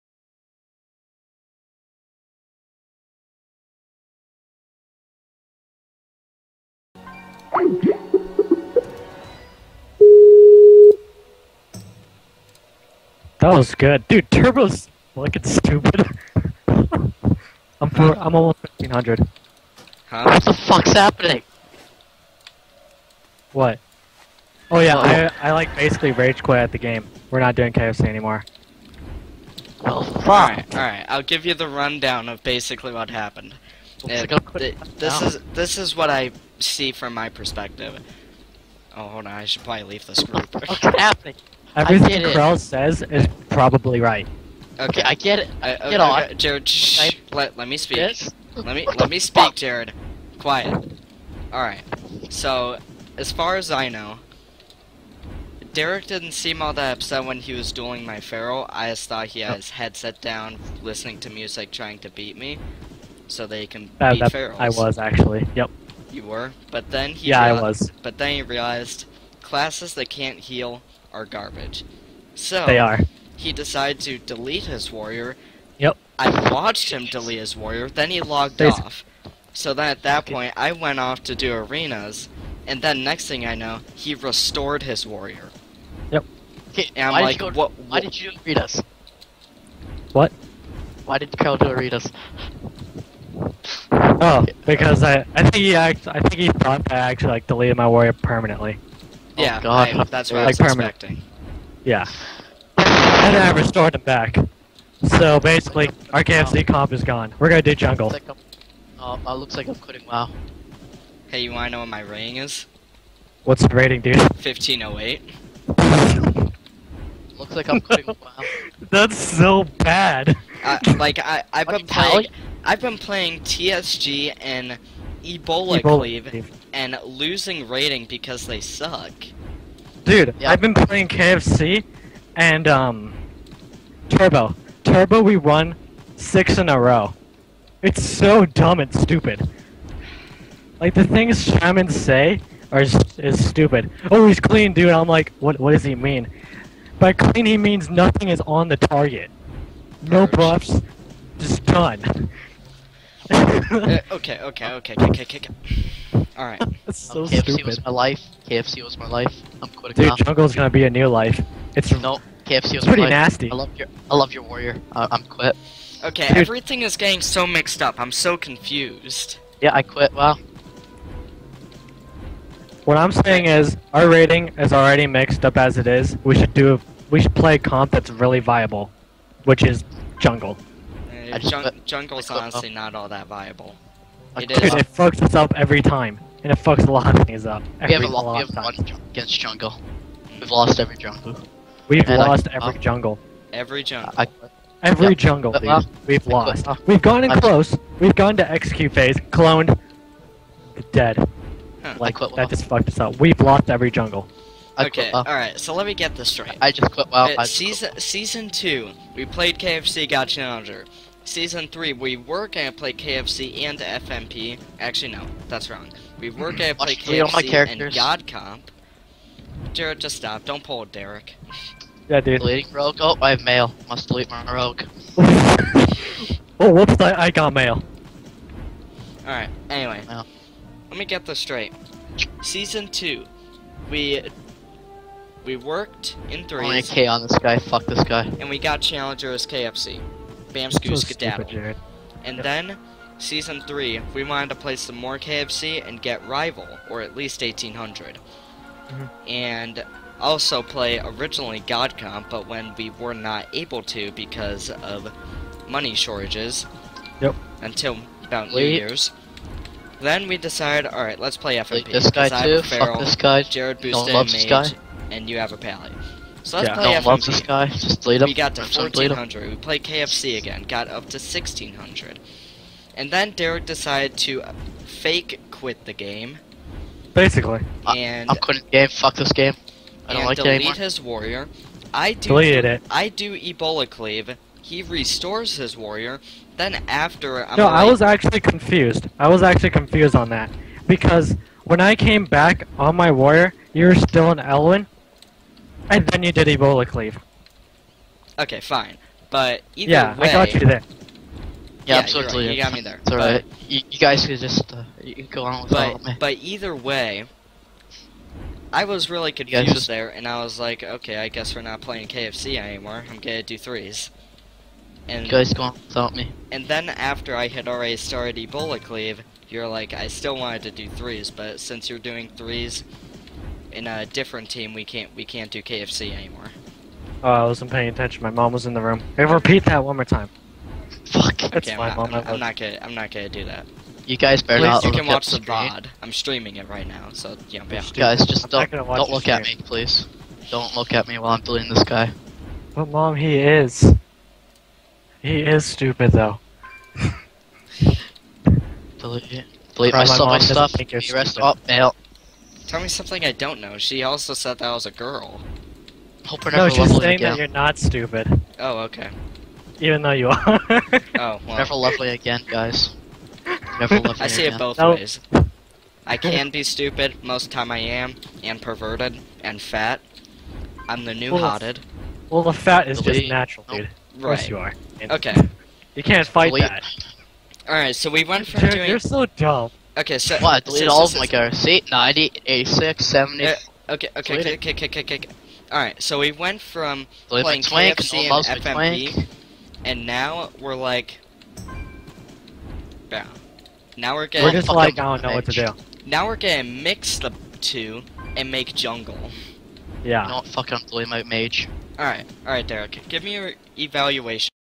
3s Comp Confusion
Derek, Jared and I got pretty confused as to what 3's comps we were going to play or if we were going to play any so this is a video of the confusion.